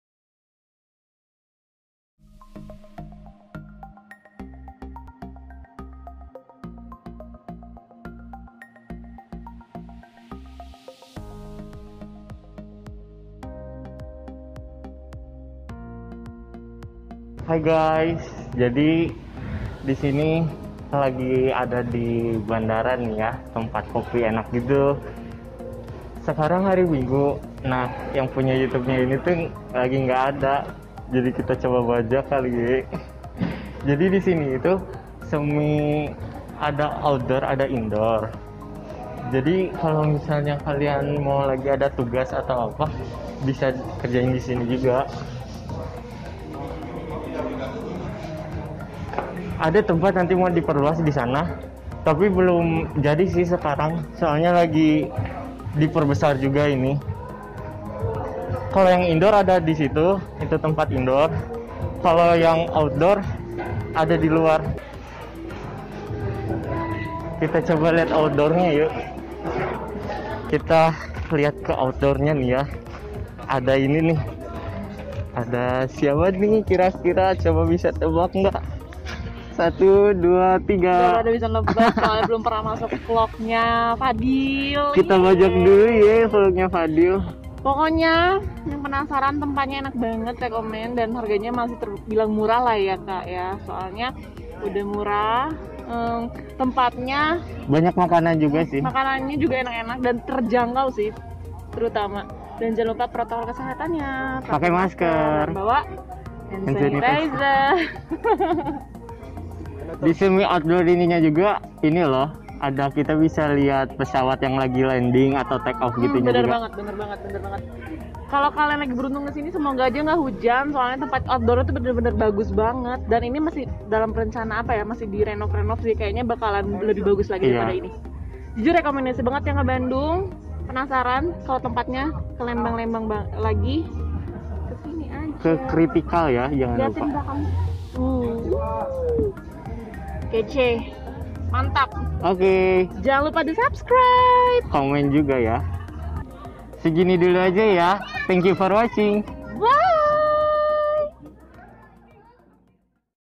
Rekomendasi Tempat ngadem di Bandung | View Bandara Husein Satranegara
Siapa yang sudah rindu liburan terbang naik pesawat? Eits, harap bersabar ya, di masa pandemi ini jangan pergi kemana-mana dulu.

Anda bisa melepas kerinduan dengan cara melihat penerbangan pesawat di kafe yang sedang hits saat ini, yaitu Critical 11.